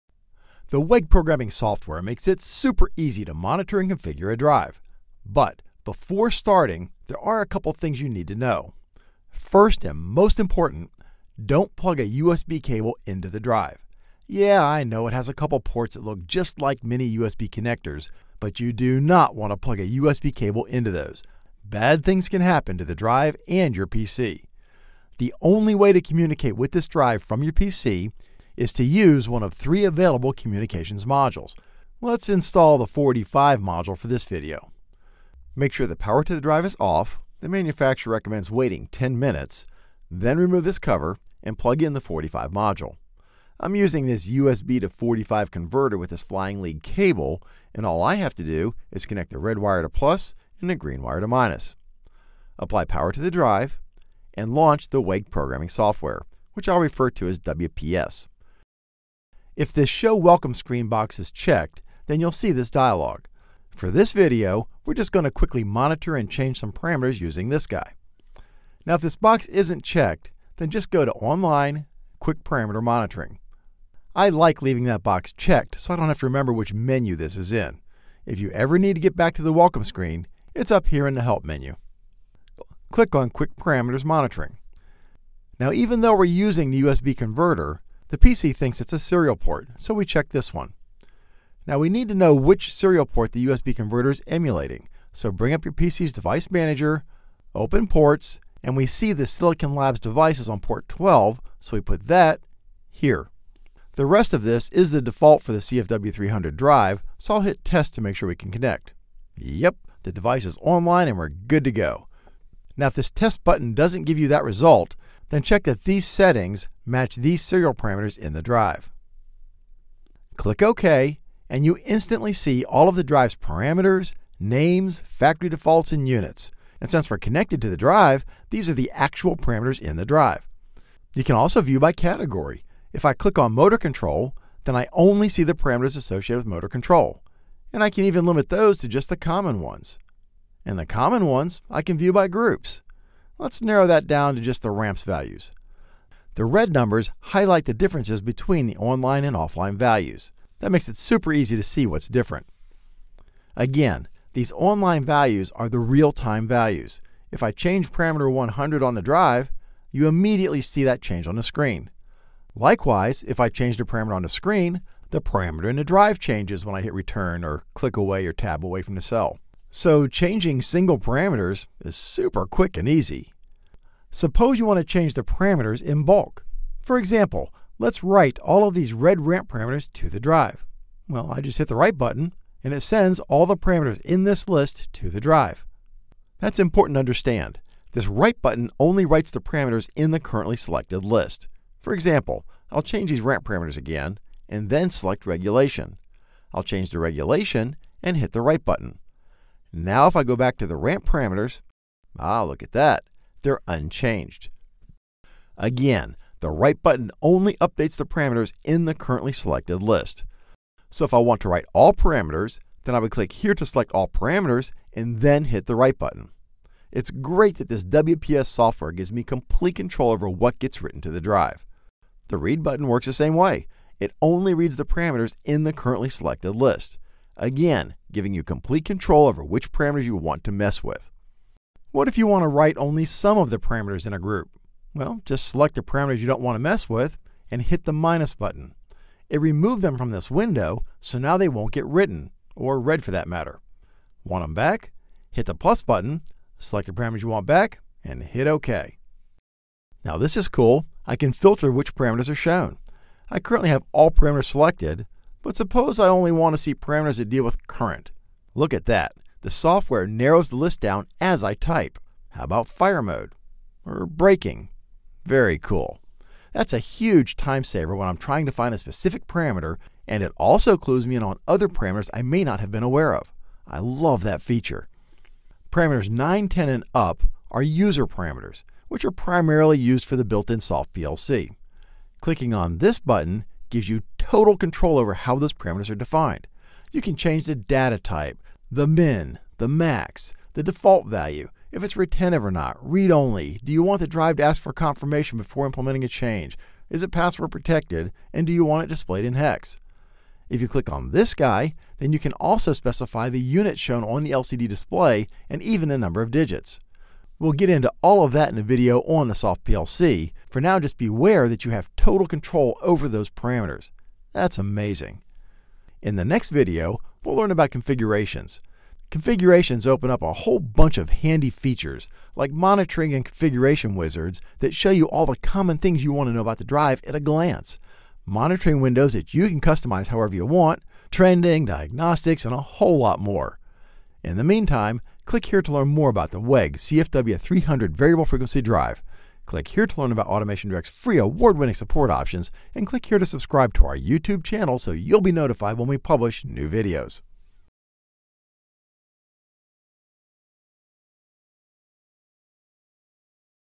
WEG CFW VFD Quick Start 1 WPS Software from AutomationDirect
A quick introduction to the FREE WEG Programming Software (WPS) and how to use it to quickly view and change parameters in a WEG VFD CFW300. For use with all WEG CFW series AC drives and SSW07 and SSW900 series soft starters. Requires PC USB or RJ45 port and corresponding communication module for CFW or SSW series products.